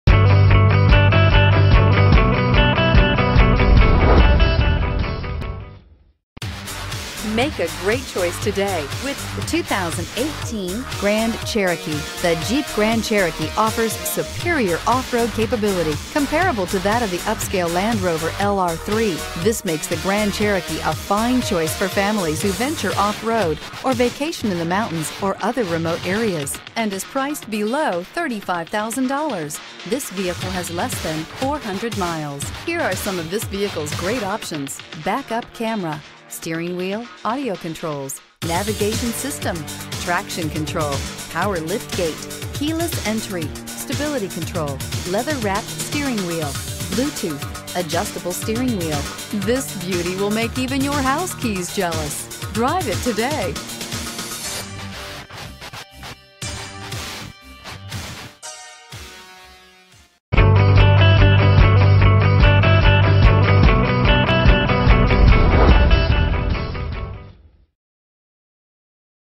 2018 Jeep Grand Cherokee Franklin TN C265355
2018 Jeep Grand Cherokee Laredo 2WD

Don't miss this 2018 Jeep Grand Cherokee. It's equipped with great features. You'll want to take this  home. Make a great choice today.